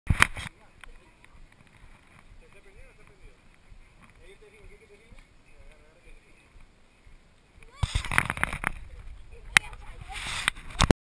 Peru Piura Frias Downhill Agosto 2016 - 09
Mountain biking from Piura Meseta Frias